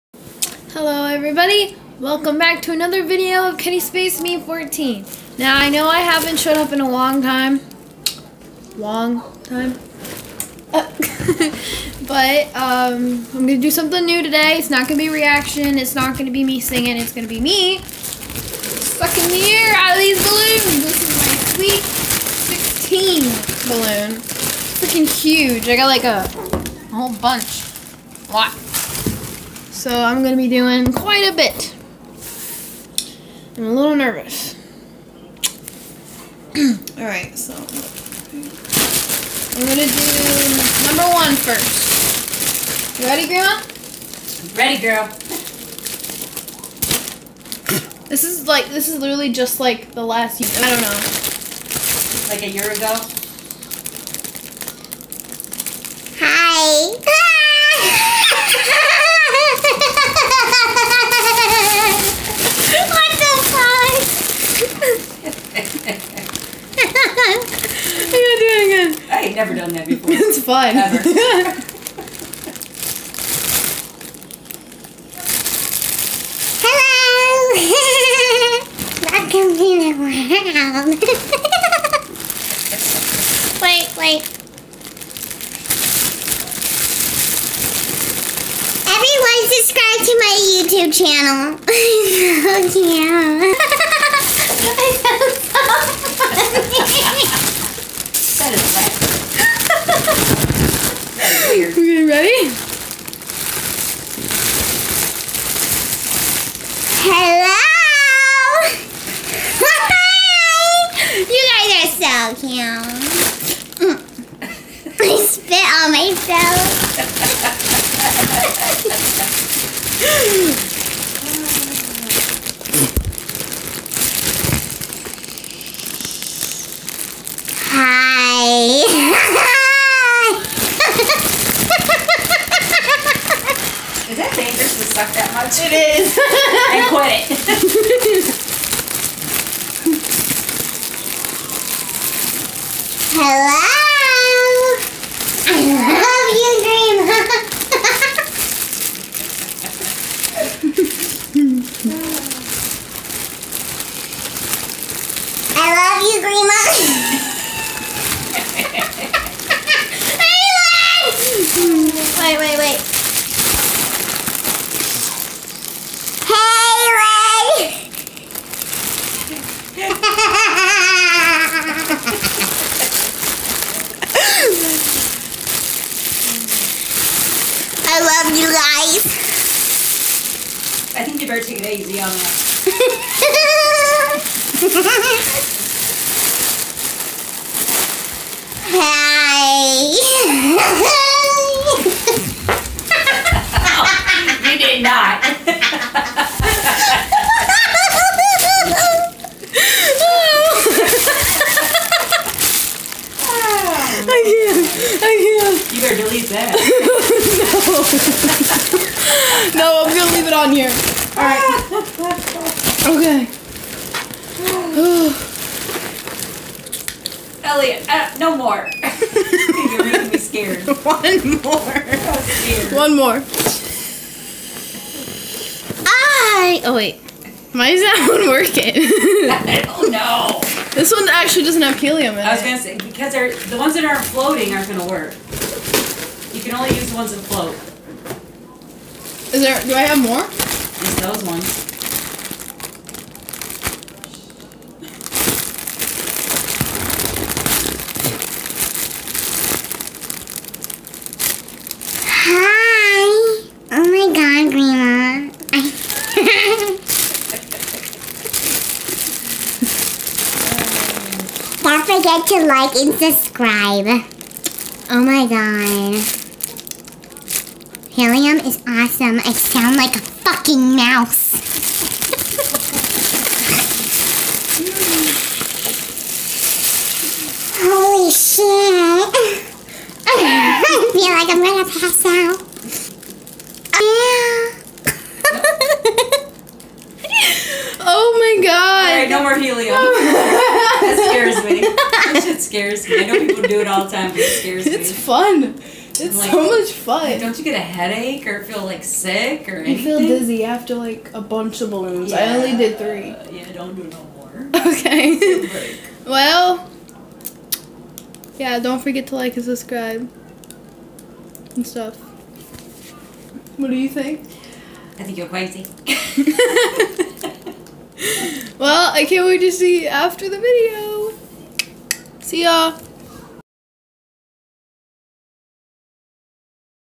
Funny😂...must watch
OMG 😂😂😂 I did the helium challenge and  the video is mest up but ohh well posting it😂😂😂hahaha